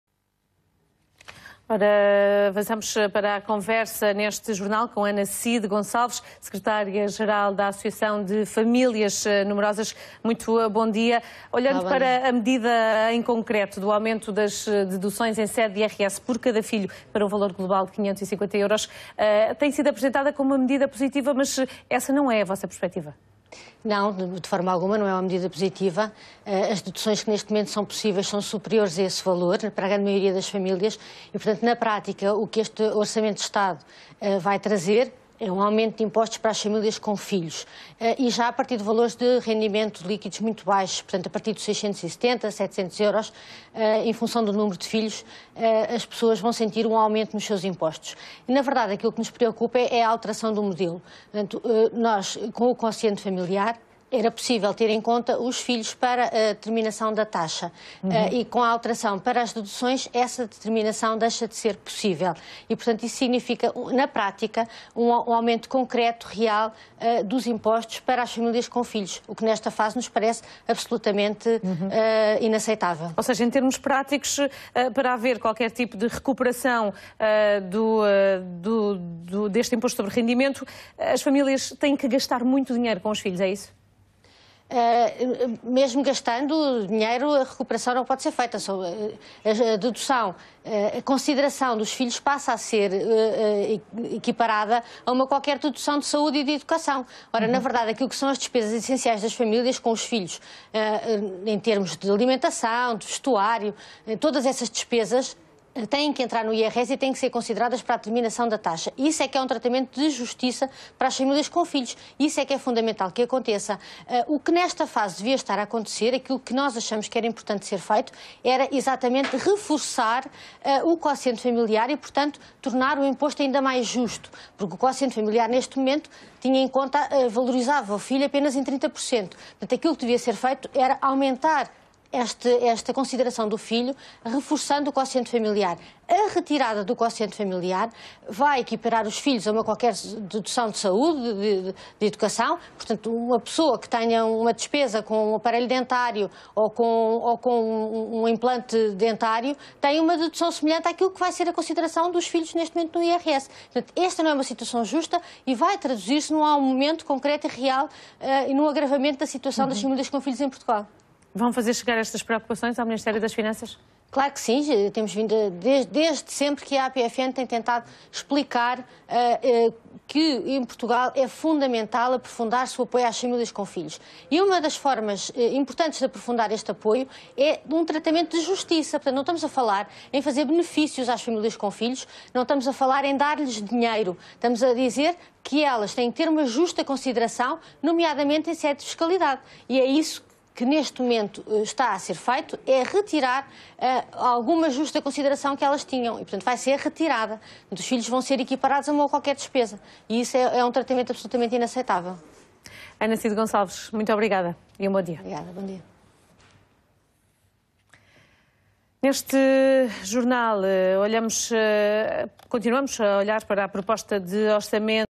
Orçamento do Estado para 2016/ Associação das Famílias Numerosas
O Governo aprovou a proposta de Orçamento do Estado para 2016.
Entrevista a Ana Cid Gonçalves, Secretária Geral da Associação Portuguesa das Famílias Numerosas.